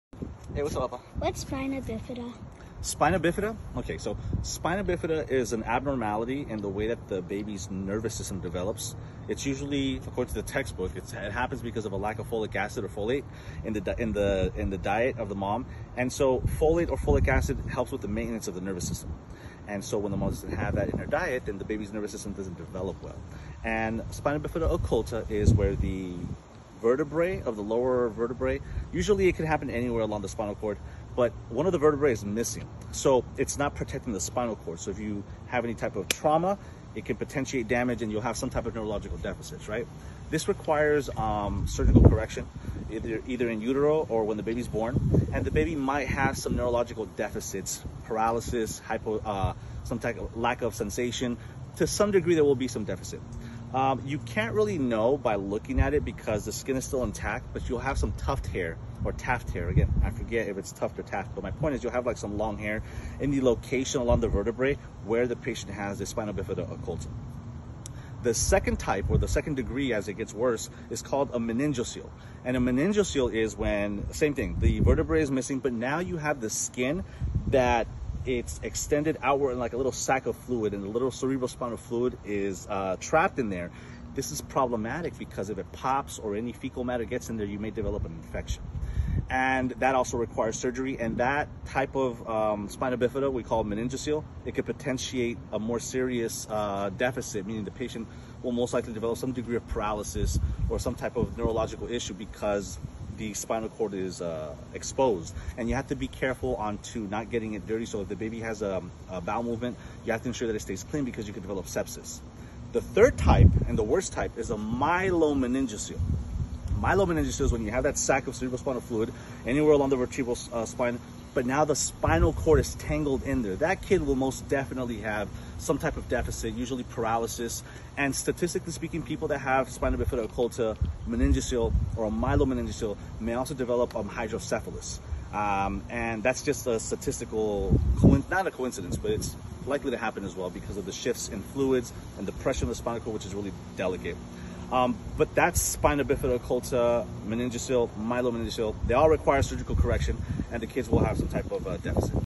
Spina Bifida Made Simple: Occulta, Meningocele & Myelomeningocele
Easily understand Spina Bifida — Occulta, Meningocele & Myelomeningocele. Learn causes (like folic acid deficiency), symptoms, treatment, and prevention. Perfect for students, nurses, or curious minds!

spina bifida, spina bifida types, occulta, meningocele, myelomeningocele, neural tube defect, folic acid, medical basics, nursing students, anatomy simplified